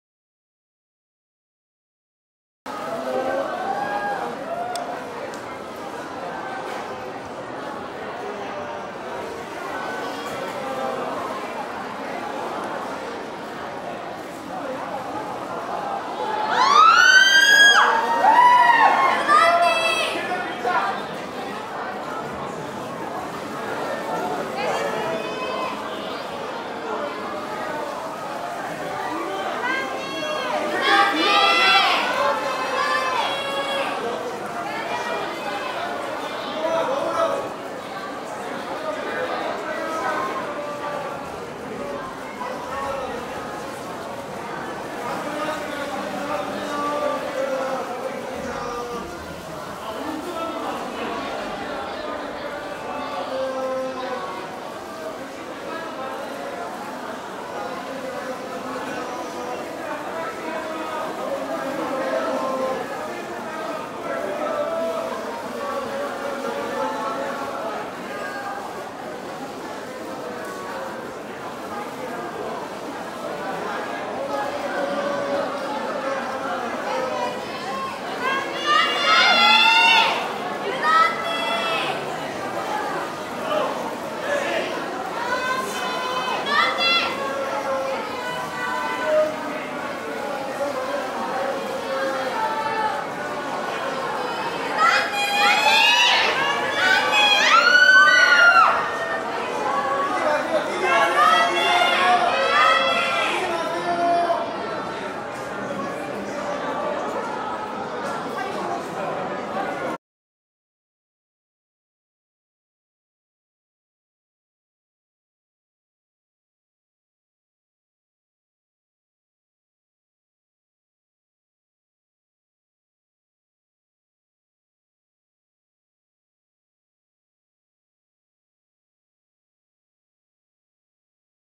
[4K] 개봉전야! VIP시사회에 공조 팀이 떴다!｜현빈 유해진 다니엘헤니 윤아 둥｜무비비
영화 공조2: 인터내셔날 VIP시사회가 열렸다.
본격적인 셀럽들의 레드카펫에 앞서
공조 주조연 배우가 팬들에게 인사하는 시간을 가졌다